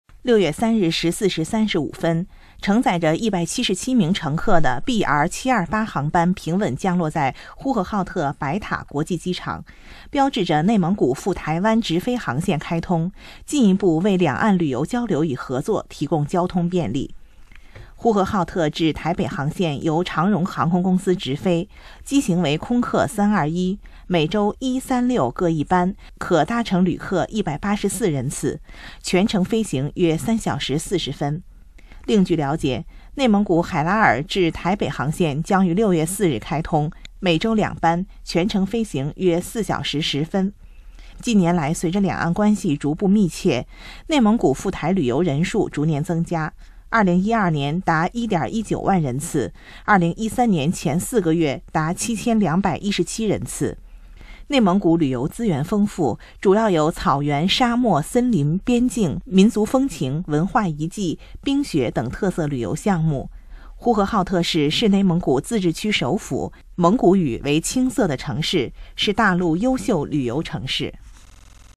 新華社》內蒙古赴臺灣直飛航線開通
新華社》 新華社呼和浩特6月3日電 ３日１４時３５分，承載１７７名乘客的ＢＲ７２８航班平穩降落在呼和浩特白塔國際機場，標誌著內蒙古赴臺灣直飛航線開通，進一步為兩岸旅遊交流與合作提供交通便利。
　　呼和浩特至臺北航線由長榮航空公司執飛，機型為空客３２１，每週一、三、六各１班，可搭乘旅客１８４人次，全程飛行約３小時４０分。
　　另據瞭解，內蒙古海拉爾至臺北航線將於６月４日開通，每週２班，全程飛行約４小時１０分，客機為空中客車Ａ３２１型，全程無中轉、經停。
　　近年來隨著兩岸關係逐步密切，內蒙古赴臺灣旅遊人數逐年增加，２０１２年達１．１９萬人次，２０１３年前４個月達７２１７人次。內蒙古直飛臺灣航班實現常態化，內蒙古「赴台８日遊」價格已由９０００元下降到８０００元以下，遊客赴臺灣旅遊成本降幅明顯。
　　內蒙古旅遊資源豐富，主要有草原、沙漠、森林、邊境、民族風情、文化遺跡、冰雪等特色旅遊項目。呼和浩特市是內蒙古自治區首府，蒙古語為青色的城市，是大陸優秀旅遊城市。